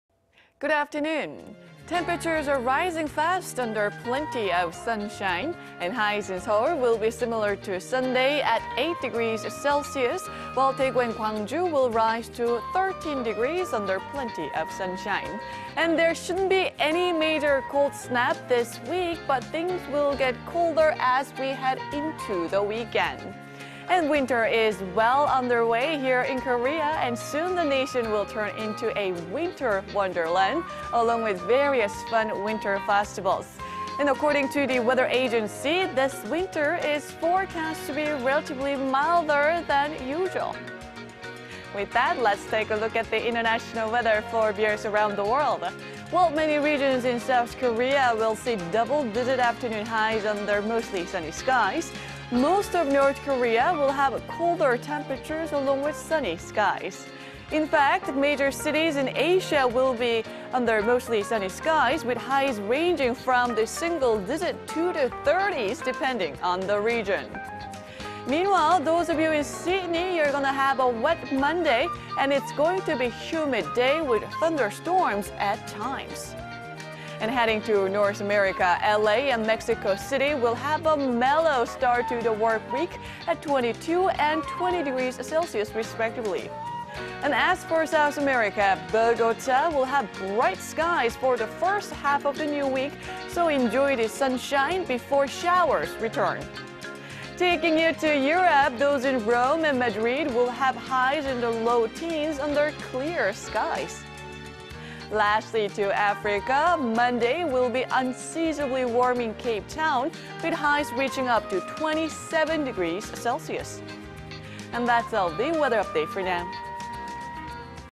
Freezing lows are rising fast under mostly sunny skies _ 112717
아침추위 낮에 풀리고, 햇빛 쨍쩅
  Good afternoon. Temperatures are rising fast under plenty of sunshine. Highs in Seoul will be similar to Sunday at 8 degrees Celsius,... while southern regions will see double digit highs... getting up to 13 degrees in Daegu and Gwangju but be aware of strong winds.

  There shouldn't be any major cold snap this week, but things will get colder as we head towards the weekend.

 Winter is well-underway here in Korea and soon the nation will turn into a winter wonderland along with various fun winter festivals. According to the weather agency, this winter is forecast to be relatively milder than usual.

With that, let's take a look at the international weather for viewers around the world.

  While many regions in South Korea will see double digit afternoon highs under plenty of sunshine, most of North Korea will have colder temperatures along with sunny skies.
In fact, major cities in Asia will be under mostly sunny skies with highs ranging from the single digits to the thirties depending on the region.

Meanwhile, Sydney will have wet Monday, it's going to be humid day with thunderstorms at times.

Heading to North America, LA and Mexico City will have a mellow start to the work week at 22 and 20 degrees Celsius.

As for South America, Bogota will have bright skies for the first half of the new week so enjoy before showers return

Taking you to Europe,...those in Rome and Madrid will have highs in the low teens under clear skies.

Lastly to Africa, Monday will be unseasonably warm in Cape Town,... reaching 27.

That's all the weather update for now.